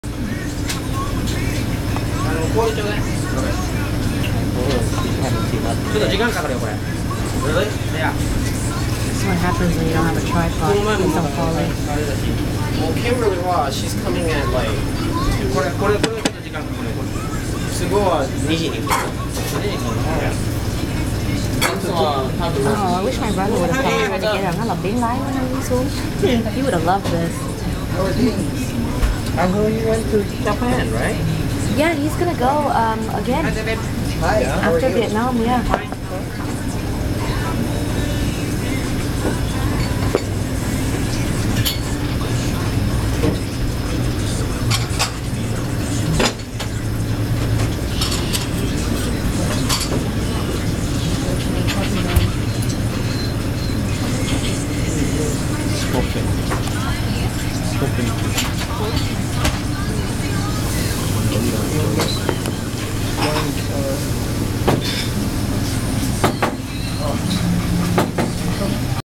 Part two of ikezukuri - scorpion fish, live sashimi
10 minutes later the fish head is still twitching